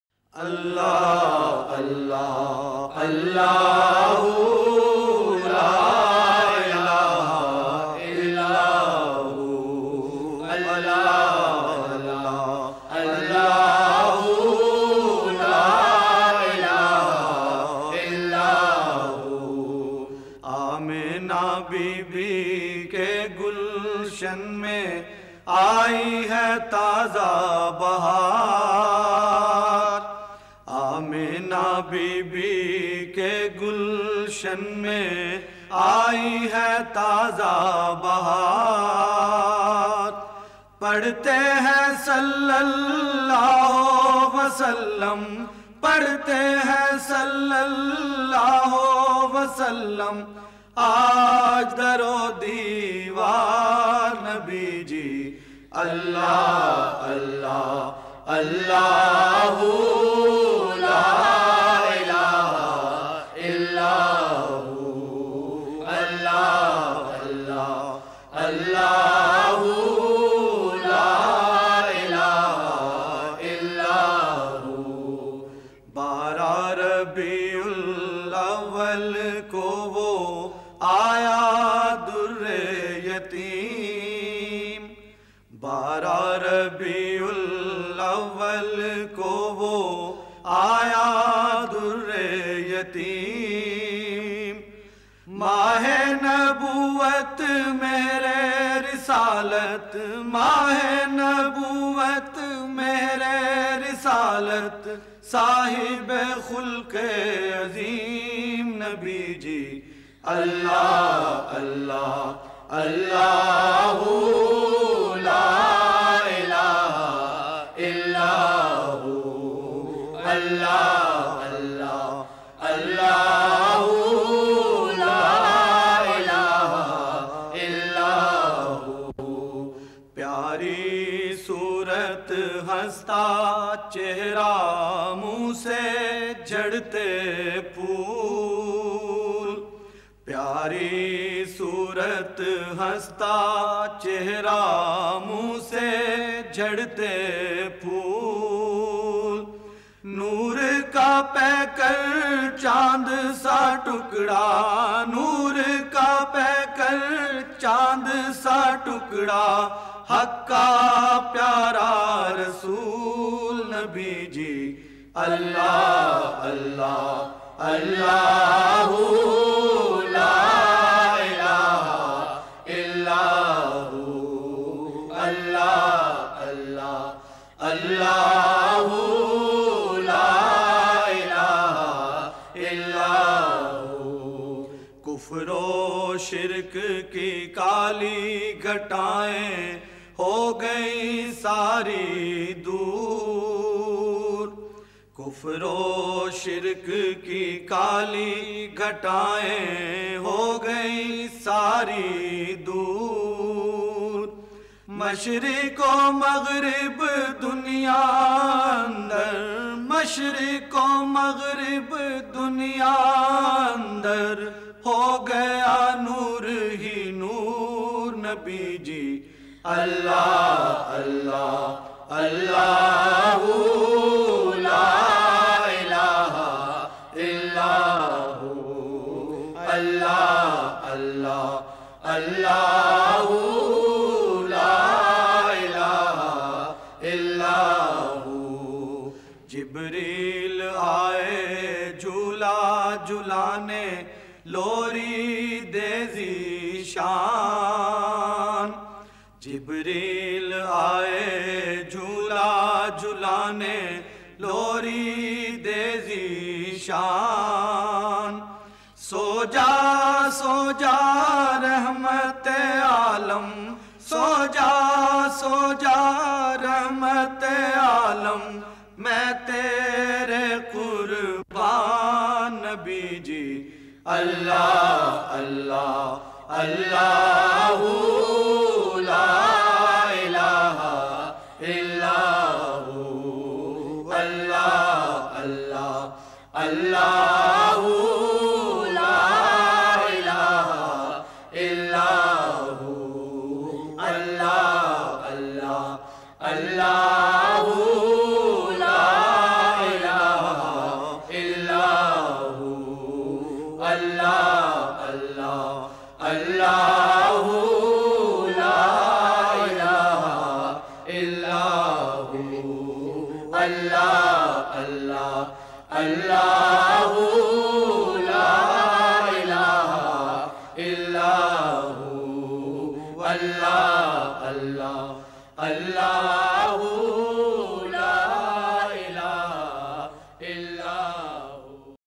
Agha Zulfiqar Khan Produced &Directed by Margoob Hamdaani Mehboob Hamdani KalamMoulana Shafi Okarvi
Naat Khwan: margoob Hamdaani , Mehboob Hamdani
Kalam: Moulana Shafi Okarvi
Produced & Directed by: Agha Zulfiqar Khan
its is PTV Production